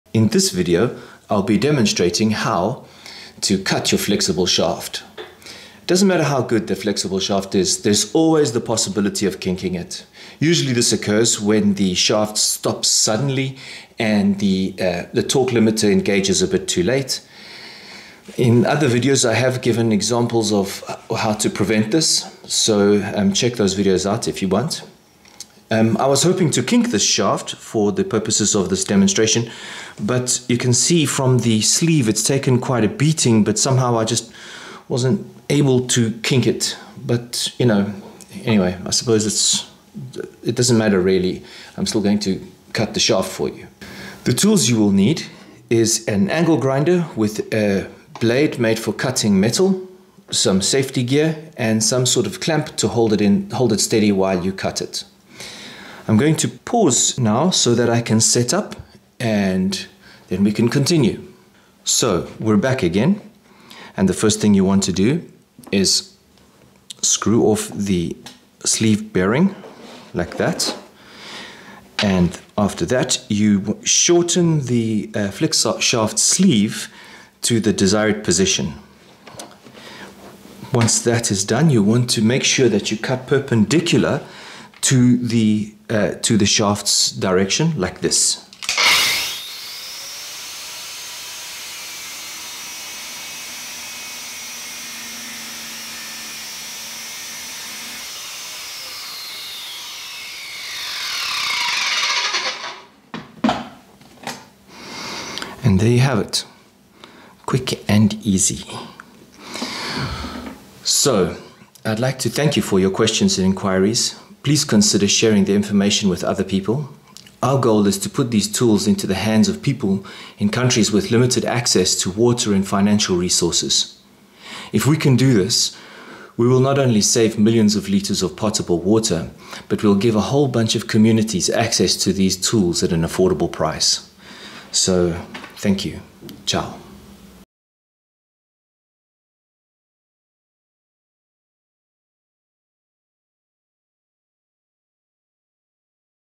How to cut a kinked flexible shaft
No matter how good the quality of your flexible shaft is, there is always the possibility of kinking it when your chain stops suddenly on a solid obstruction, and the torque limiter does not engage in time.

This can be prevented by reducing the space between the sleeve bearing and the drill adapter that connects your flexible shaft to your drill.

This video describes the process of cutting a flexible shaft after it has kinked. It is a very simple process, but it can save you so much trouble, money and time.

Few important things to remember:

Please be sure to lubricate your flexible shaft before the first use, and then check the lubrication every few weeks. This not only ensures the longevity of your shaft, but also makes it more efficient over longer distances. Our preference for lubricant is basic machine oil. Here is a video we made on how to lubricate your shaft

Feel free to choose between a corded or a cordless drill, but make sure not to use the hammer action function on the drill because it can cause damage to your shaft.

With a flexible shaft you will be using less than 3% of the water you would need to get the same result with a hydro jetting machine. Imagine the amount of water you will save by using a flexible shaft.

0:00 What equipment and tools you will need
1:01 Cutting the flexible shaft
1:50 Importance of what we do

Important note: We manufacture our own brand of shafts, and by chance our clients started adapting them to their Ridgid K102 or K204 sewer cleaning machines. They also informed us that our flexible shafts are a bit more rigid than the Ridgid flexshafts. We do not claim that we have a replacement shaft for a Ridgid Flexshaft.